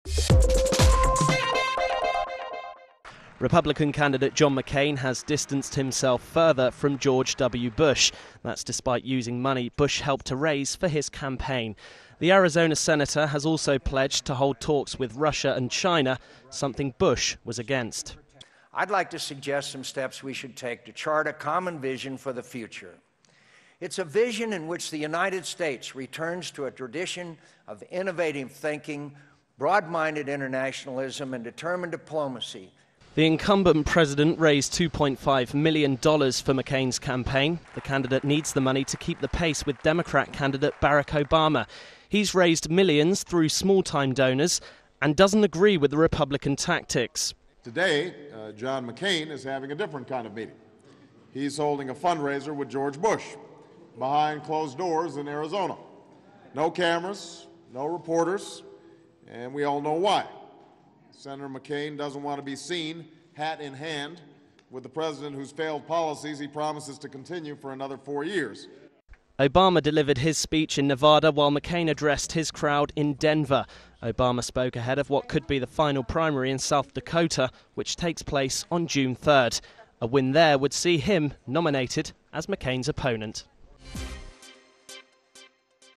McCain V Obama
Republican candidate John McCain has distanced himself further from George Bush. That's despite using the President's money for his campaign. Test